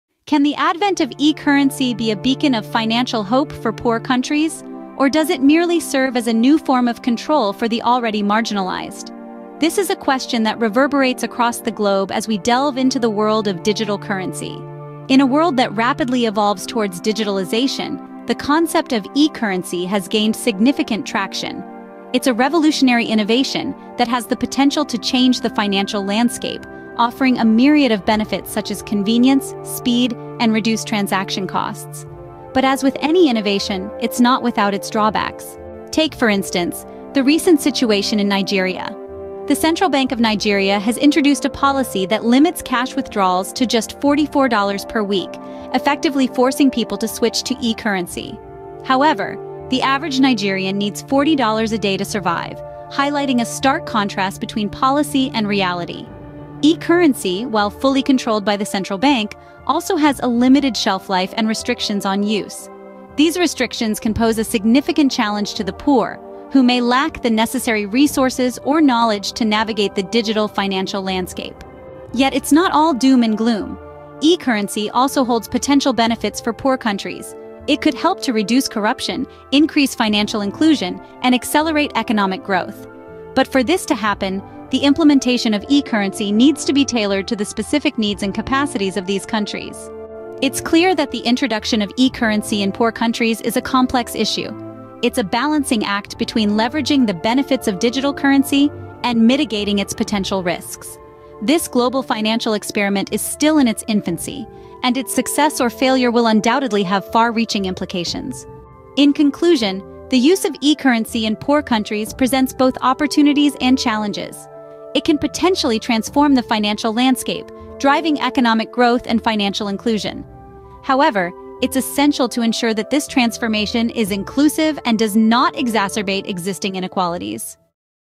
E-Currency Trials in Developing Nations: A Case Study of Nigeria
In this insightful video, we delve into the fascinating topic of e-currency trials in developing nations, with a specific focus on the compelling case of Nigeria. Join us as we explore the challenges, opportunities, and potential impacts of adopting digital currencies in emerging economies. From examining the motivations behind this trend to analyzing the outcomes witnessed so far, we leave no stone unturned in uncovering the potential future of e-currencies in developing nations. Together, let's unlock the true potential of e-currencies and their role in shaping the world.